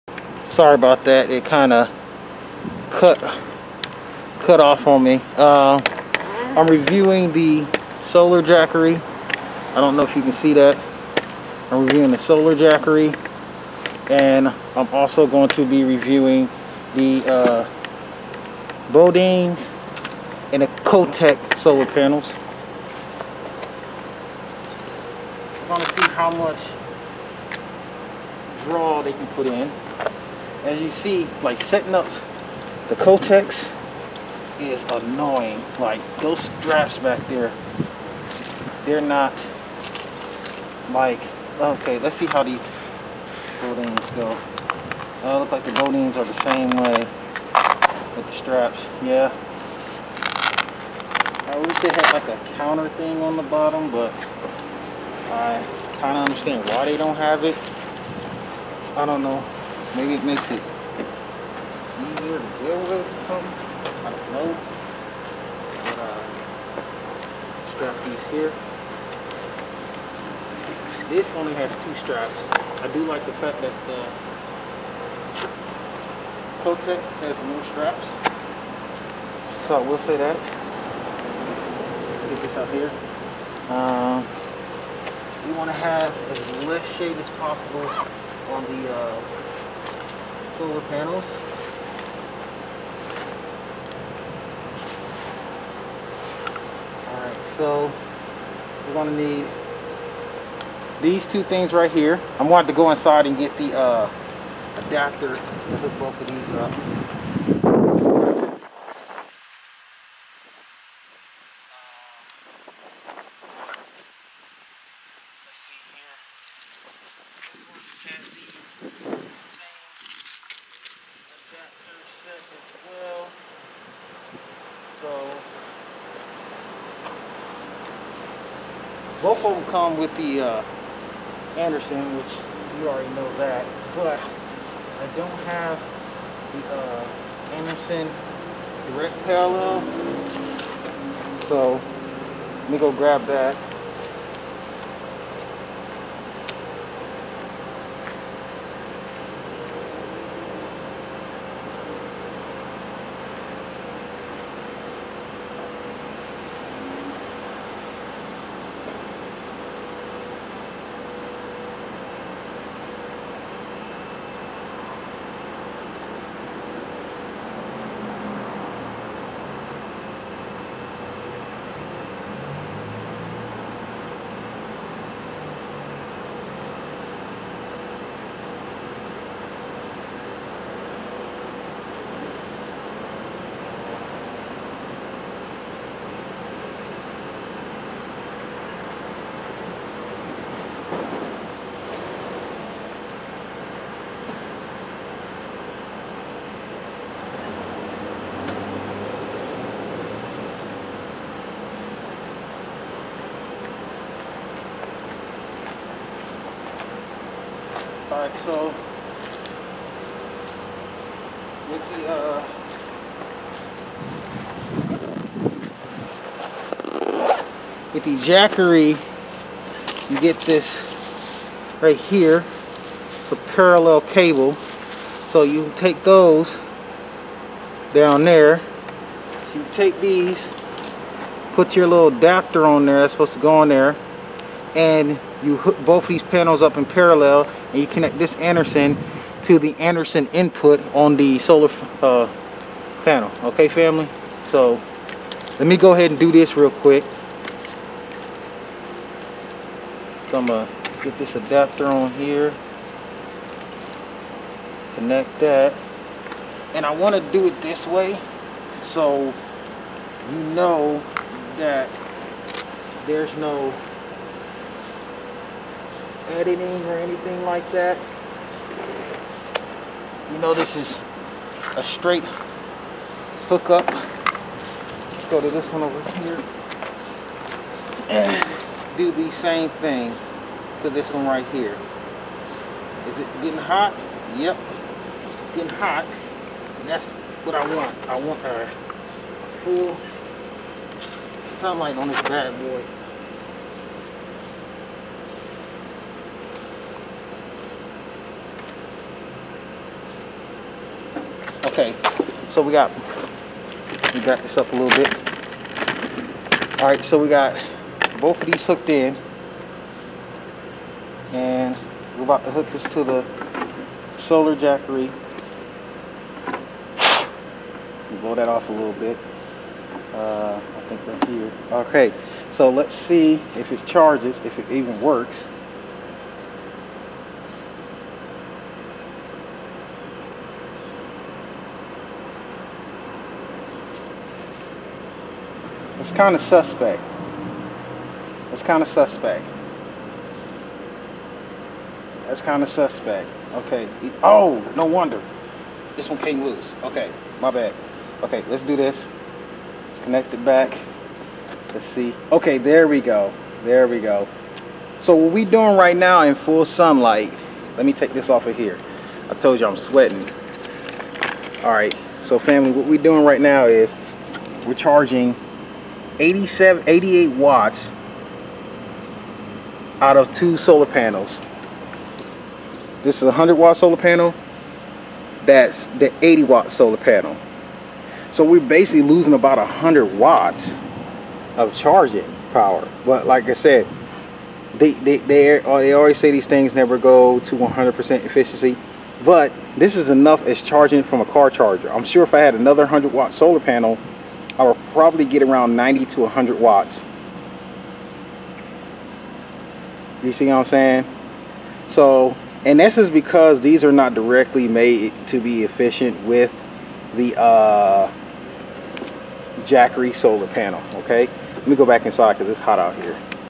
Beaudens and Cheotech Solar panels with Jackery 1000 test
Beaudens is 100W while Cheotech is 80W but was only able to in complete sun light get 88W and average charge rate. But then again these are 2 different solar panels and im surprised it worked at all lol